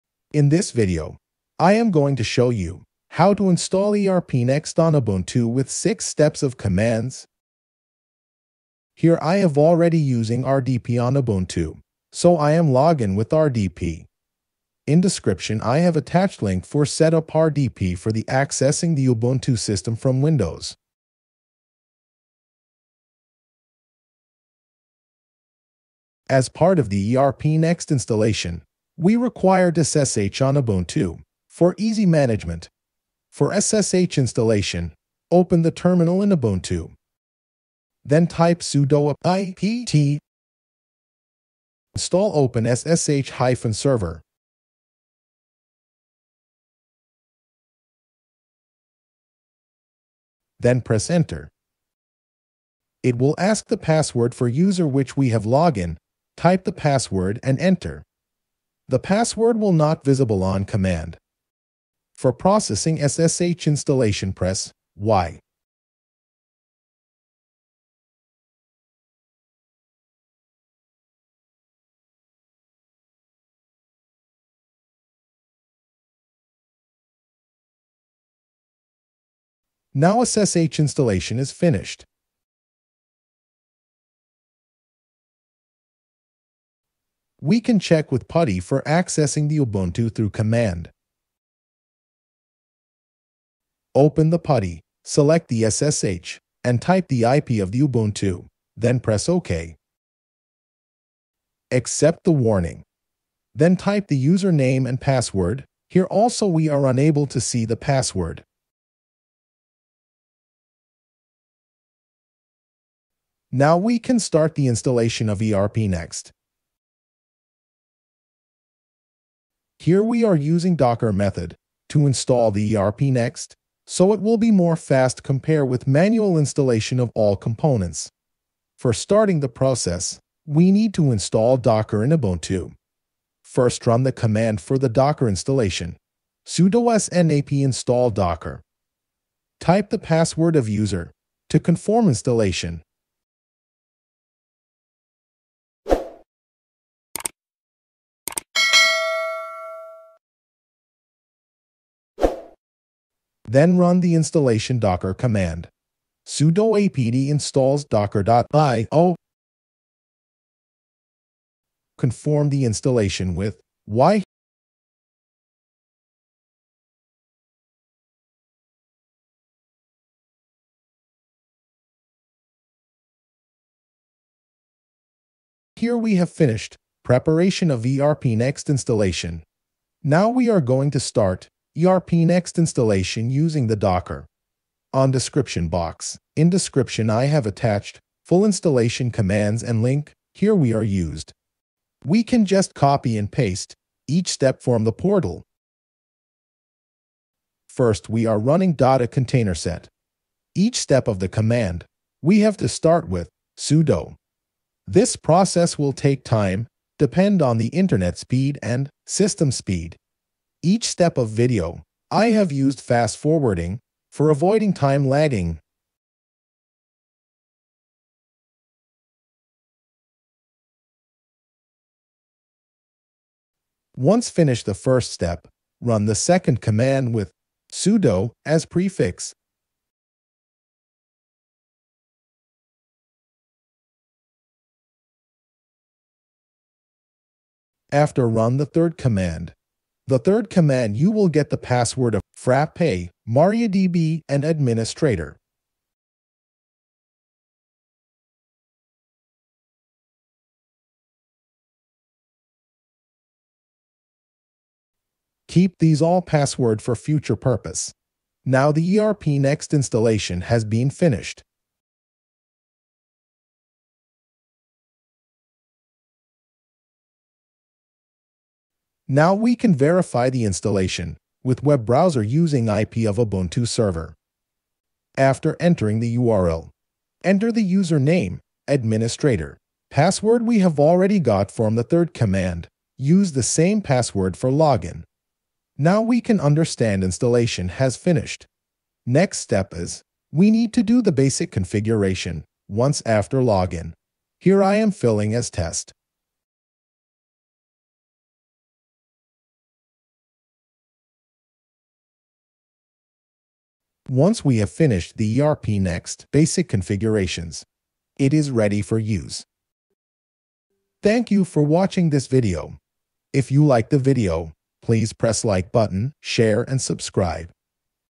how to install erpnext on ubuntu
In this video will show you howtoinstall erpnext on ubuntu , This video will show you how to install ssh on the ubuntuserver for the erp next installation . This is the installation we are using the Docker mthode for fast installation . In this video will also help to show installation of docker in Ubuntu .

Use the below command .

sudo apt openssh-server
sudo sanp install docker
sudo apt install docker.io
sudo docker create -v /home/frappe/frappe-bench/sites/site1.local/ -v /var/lib/mysql --name erpdata davidgu/erpnext
sudo docker run -d -p 80:80 --name erpnext --volumes-from erpdata davidgu/erpnext
sudo docker exec -ti erpnext cat /root/frappe_passwords.txt Login with Administrator / password

RDP installation reference link for Ubuntu

00:00 : How to install Erpnext on Ubuntu
00:30 : How to install SSH on ubuntu
01:30 : How to access ssh using putty from windows
02:06 : How to install docker on Ubuntu
03:10 : starting erpnext installation
04:30 : First login of erp next
04:49 : Basic configuration of Erp Next
05:19 : Outro